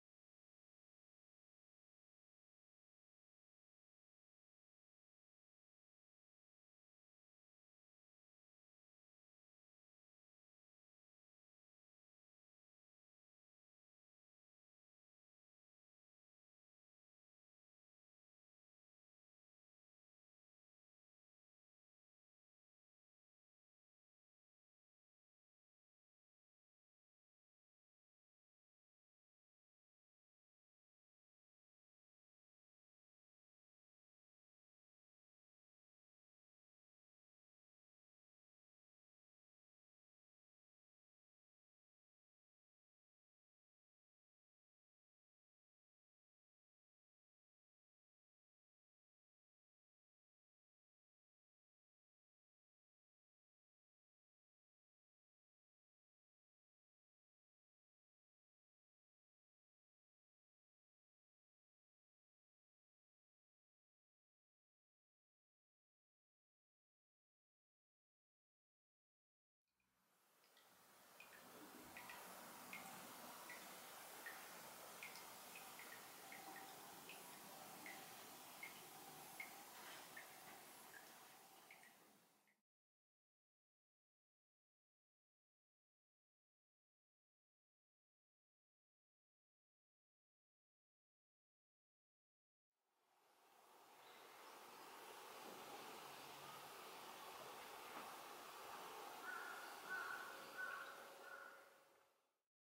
京都の庭園・芬陀院／雪舟寺 The Garden of Kyoto Funda-in Temple Sesshu Garden. Cool Japan Full HD Movie
Full HD Movie The Garden of Kyoto Funda-in Temple Sesshu Garden
東福寺塔頭の芬陀院／雪舟寺です。

京都 庭 名庭